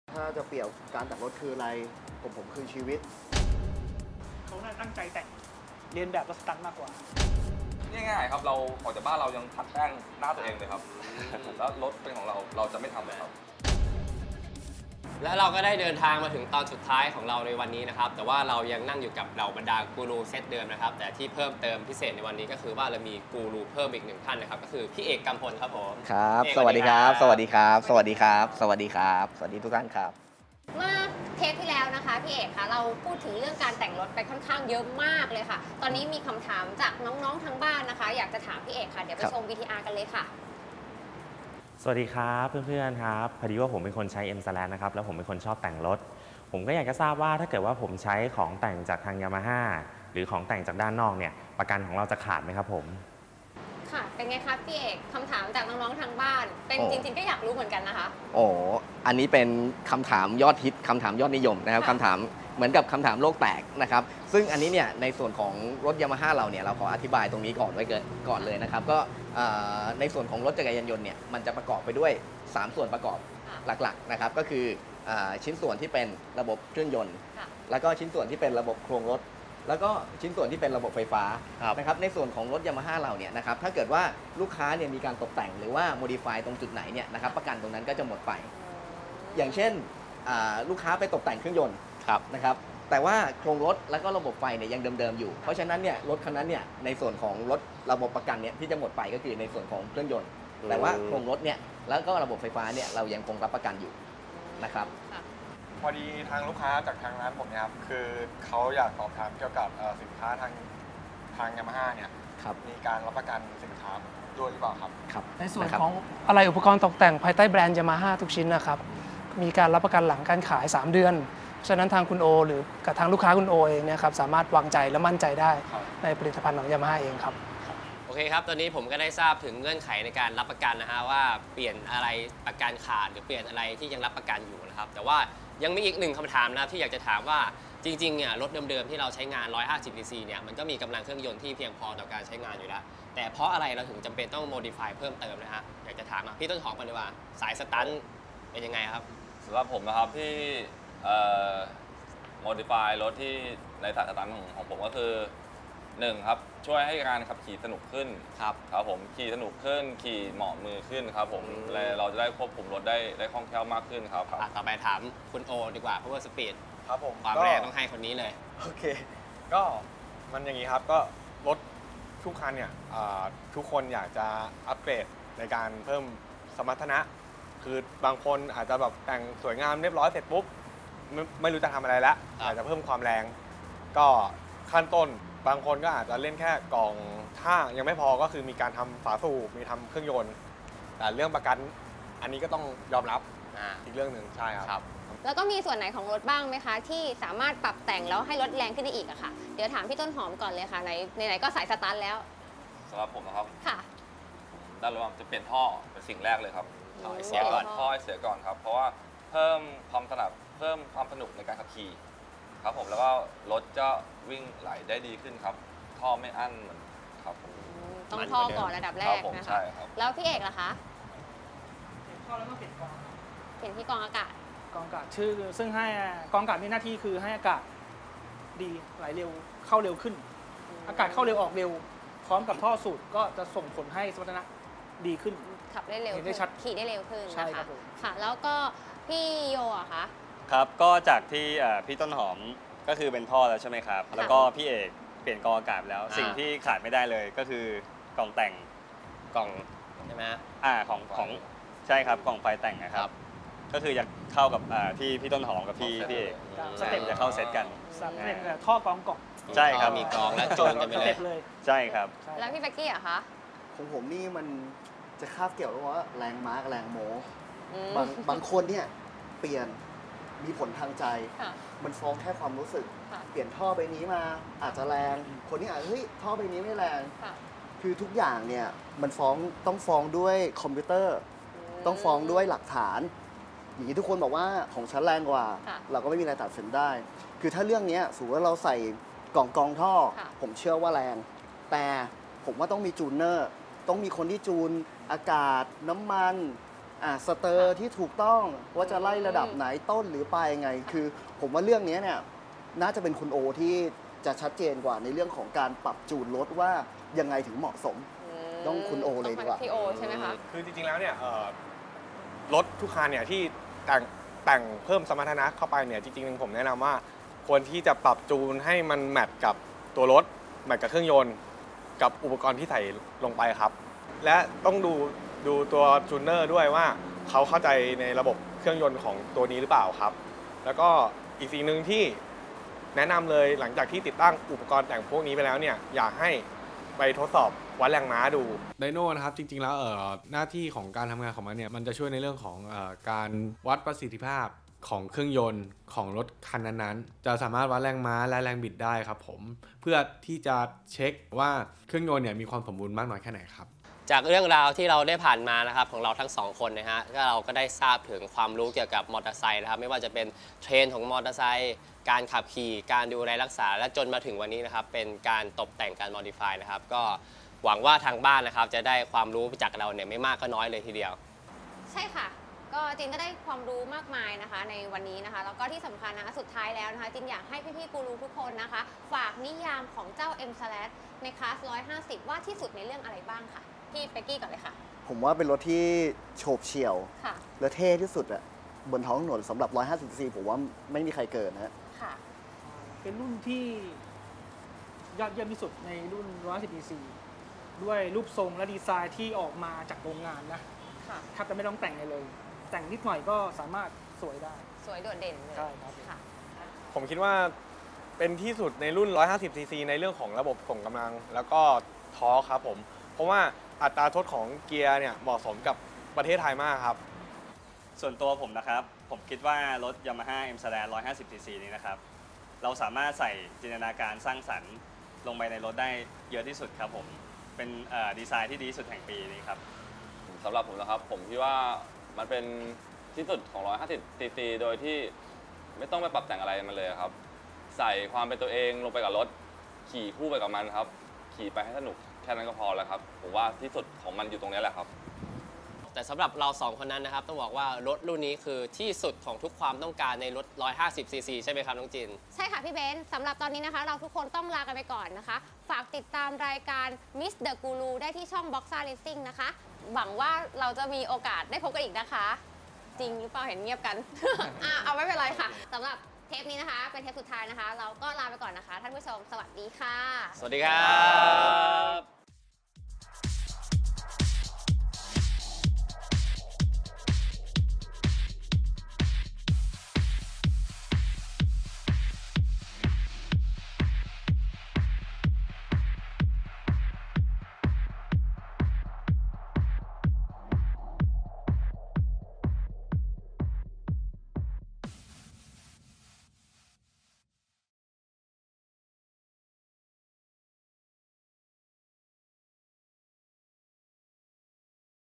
M-SLAZ BoxzaRacing Meet The Guru : Chapter 4 Part 2: Design And Custom Trend
รายการ BoxzaRacing Meet The Guru Chapter 4.2 เดินทางมาถึงตอนสุดท้ายแล้ว กูรูทั้ง 6 ท่านยังคงอยู่เหมือนเดิม เพิ่มเติมคือ มีอีกหนึ่งกูรูจาก Yamaha ที่จะมาไขข้อข้องใจยอดฮิตอย่าง แต่งรถแล้ว ประกันจะขาดหรือไม่ ละอีกหลายๆคำถาม ซึ่งคำตอบอยู่ในคลิปแล้ว ไปชมกันเลย !

รายละเอียดเพิ่มเติมพร้อมภาพสวยๆ ชมได้ที่

BoxzaRacing.com เว็บไซต์ยานยนต์ อันดับ 1 ของประเทศไทย